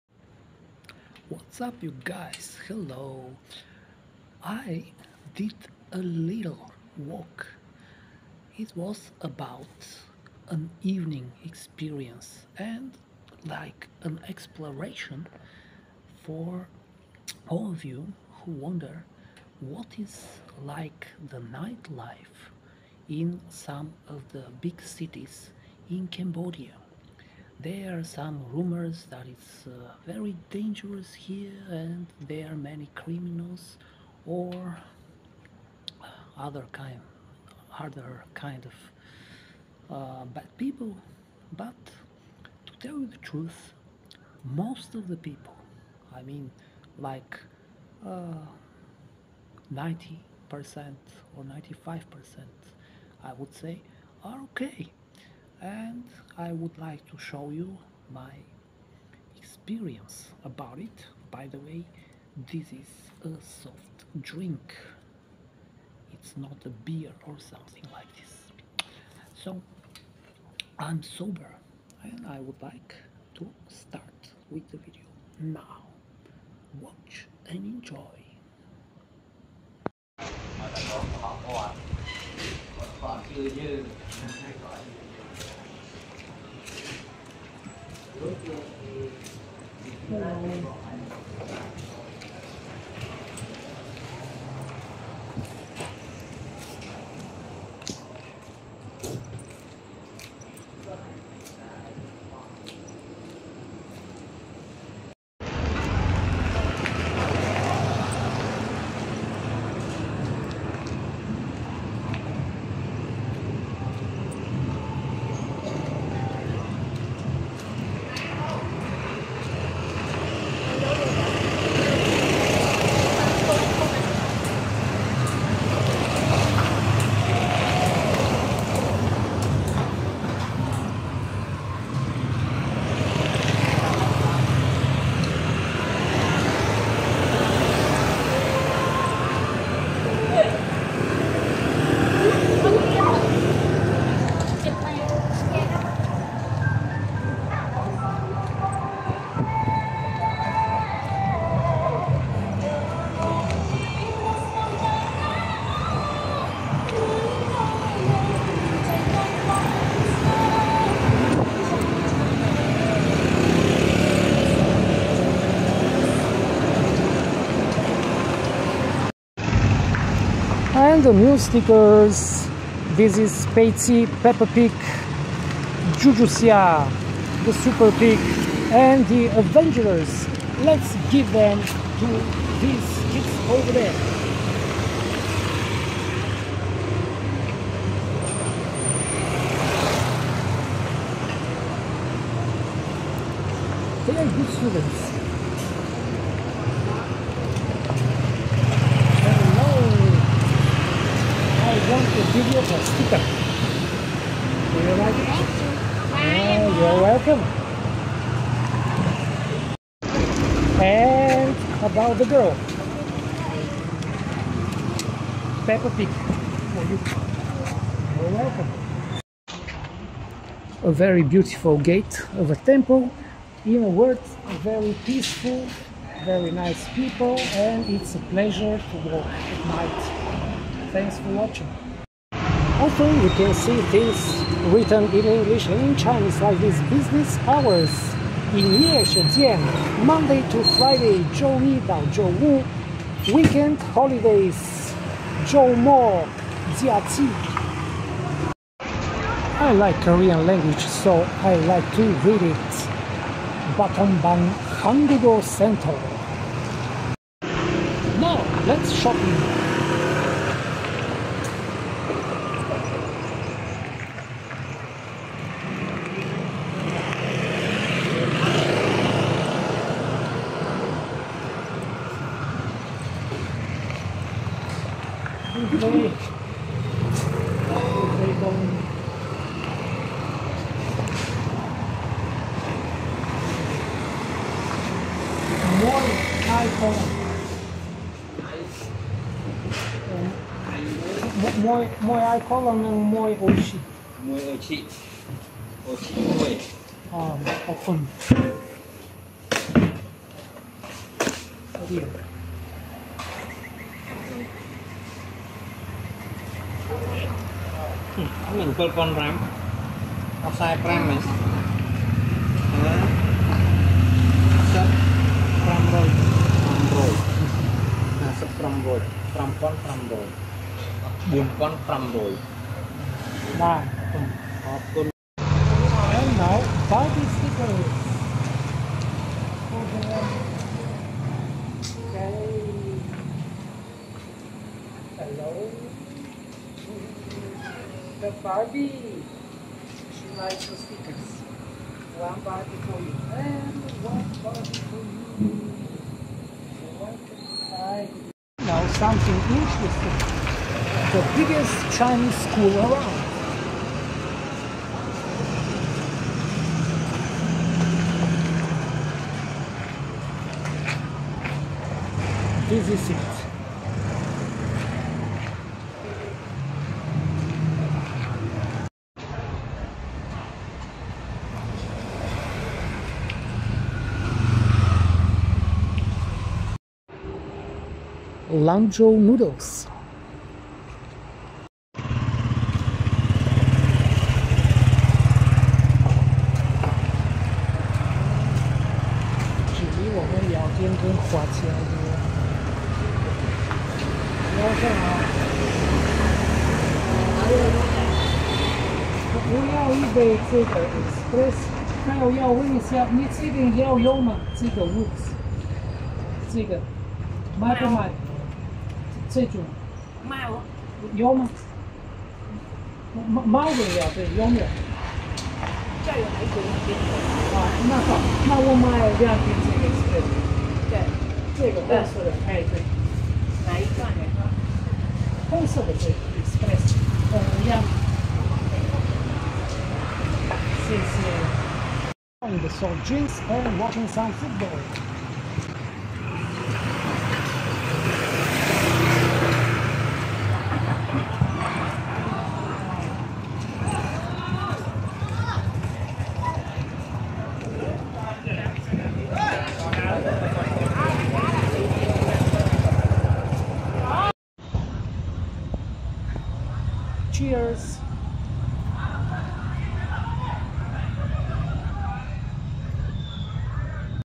What is the night life in a Cambodian city like?
Just a frank opinion and some simple experience.